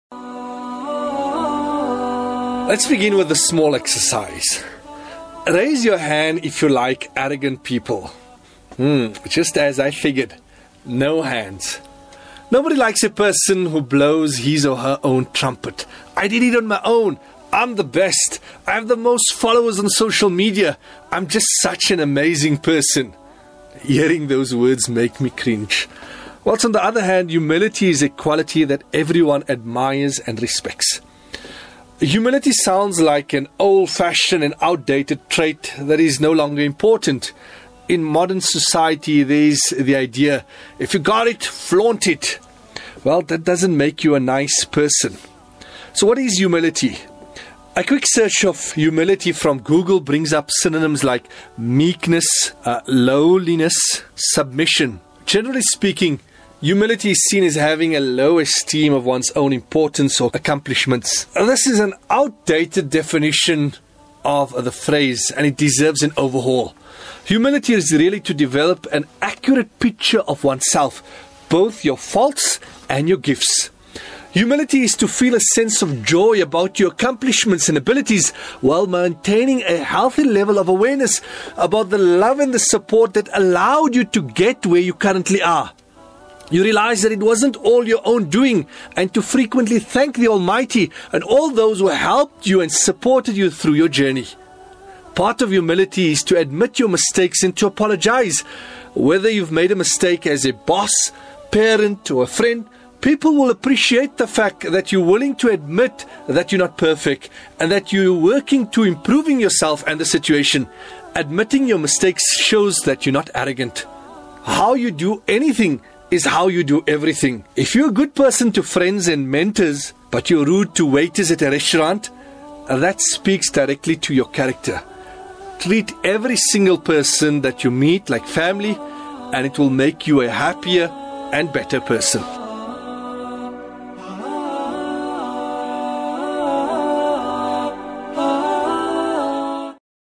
Be Humble | Mufti Yusuf Moosagie
Be Humble

No matter how high up you get, never forget where you came from.
A mistake that makes you humble is better than an achievement that makes you arrogant.
Be patient when you have nothing and be humble when you have everything.

Short clip by Mufti Yusuf Moosagie